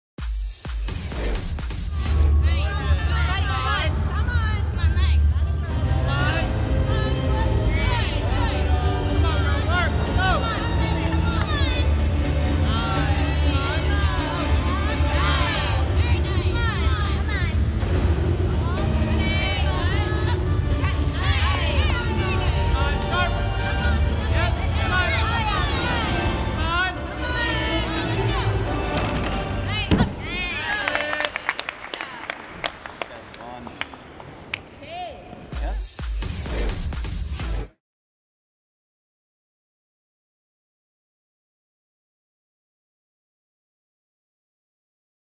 Aurelie Tran (CAN) - UB - Podium Training - 2023 World Championships Antwerp
Aurelie Tran of Canada on uneven bars during podium training at the 2023 World Championships in Antwerp, Belgium.